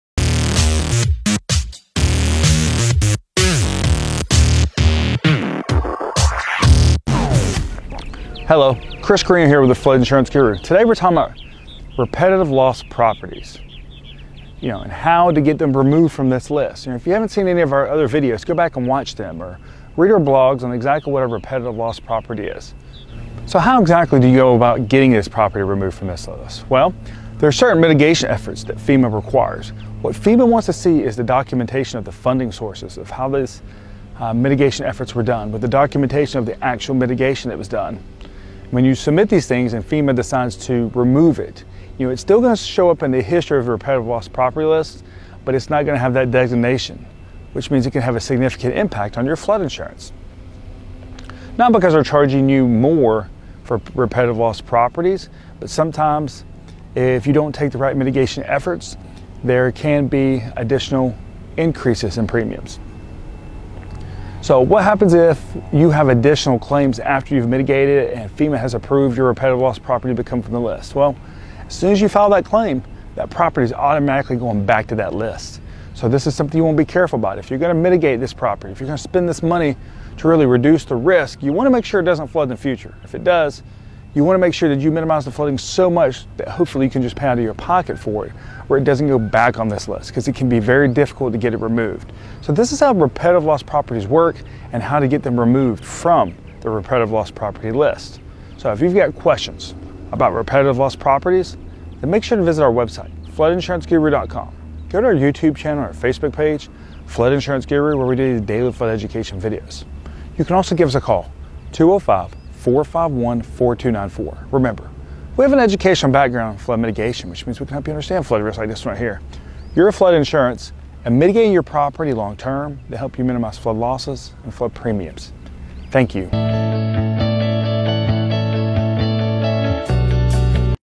Flood Insurance: Repetitive Loss Properties and How to Get Them Removed
In this video we look at flood insurance and repetitive loss properties.